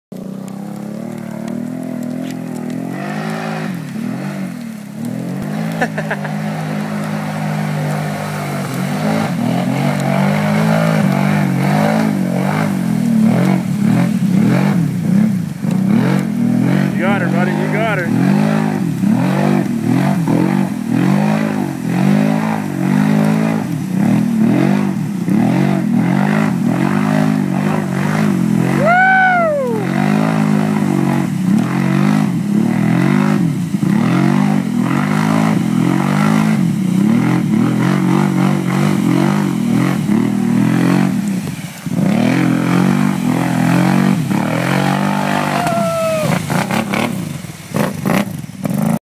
Brute Force 750 Mudhole
Piped and cammed Brute going through a decent mudhole in the rain